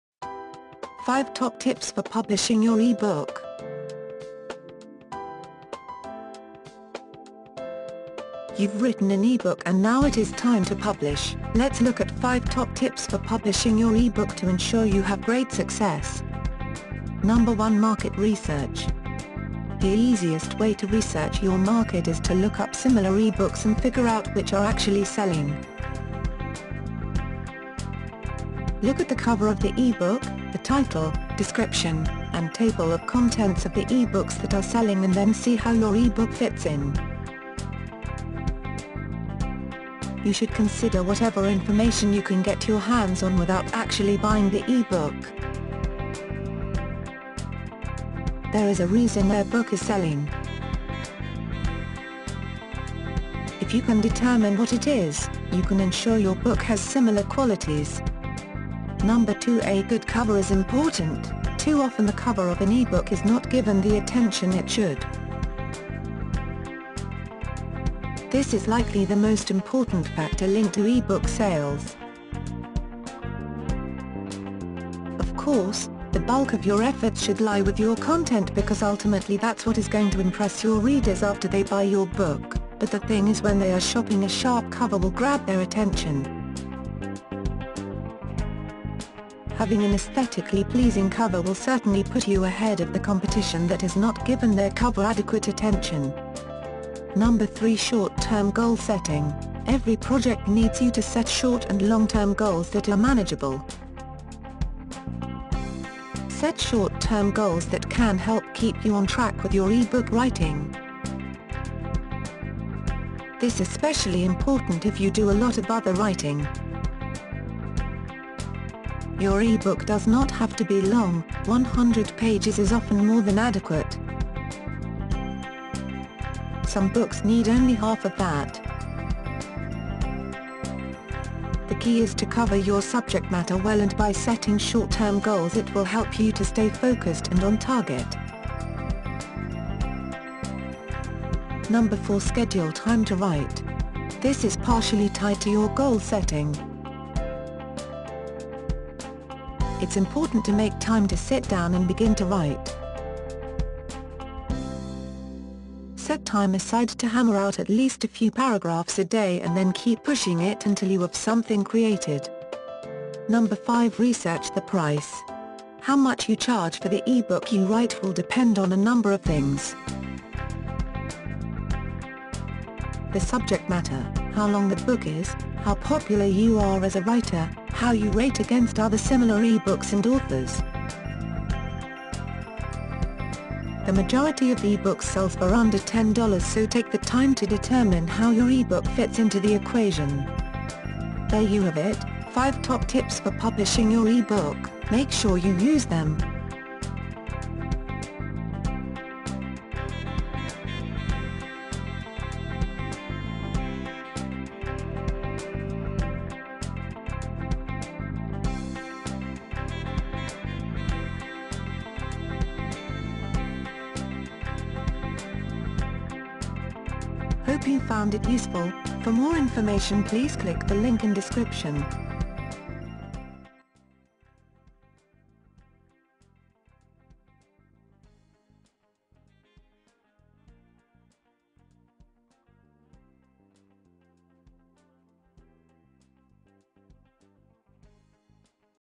5 Top Tips for Publishing Your E-book
You’ve written an ebook and now it is time to publish. Let’s look at 5 top tips for publishing your ...